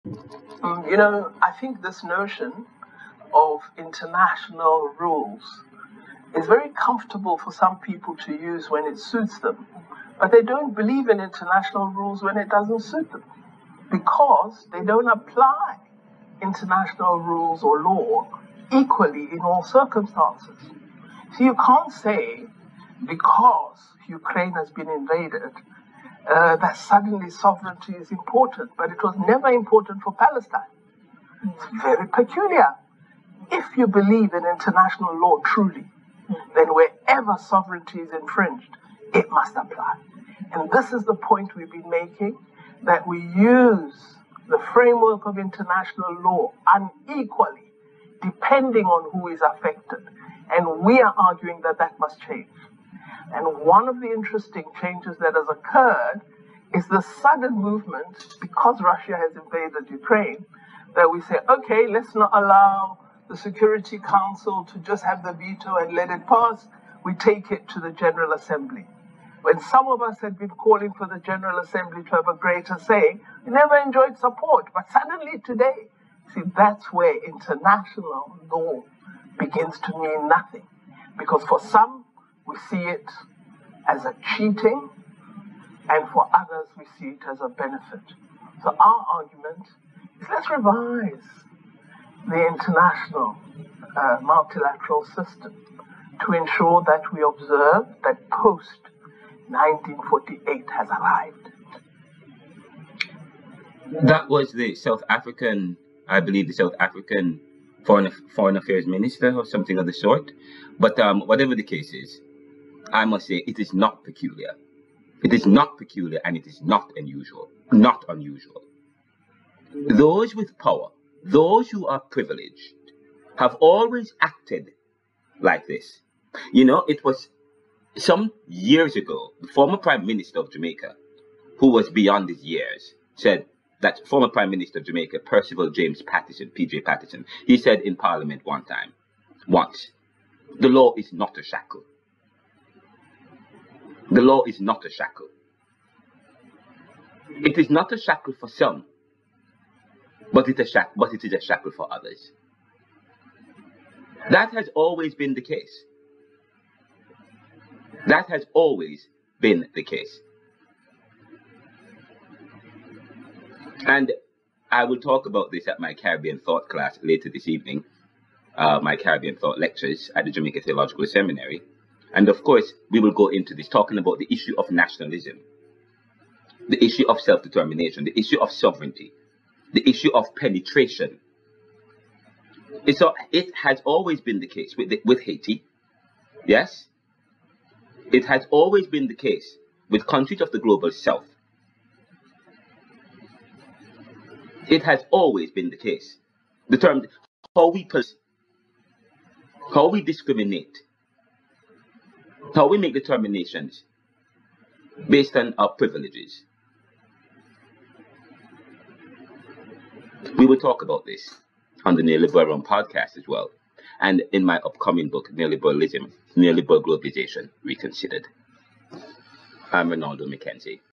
Neoliberal Globalization Reconsidered: South African Foreign Minister Comments on International Law
This is powerful. Why? In 1947-48 (after the 1944 UN formation where colonies were absent,) Israel invaded and occupied Palestine and became a nation much to the protestation of Palestine. Did the usa support the Palestinians against this invasion? Now russia is invading ukraine, is the US supporting Russia or Ukraine? Sarah Abdallah. BTW, Former PM of Jamaica PJPatterson said "the law is not a shackle..." This was always the case in a competitive world based in privilege where some are above the laws and others below it. Who are those above the law? I discuss this tonight in my Lecture at caribbeanthought Jamaica Theological Seminary and in my upcoming and 2nd academic textbook neoliberalglobalizationreconsidered: Neo-Capitalism and the Death of Nations and on theneoliberalround podcast.

Man's Laws based and Man's chauvinisms.

BTW, you may participate in our podcast:
Question on International Law; should the US have supported the Palestinians against European invasion leading to the takeover parts of Palestinian State?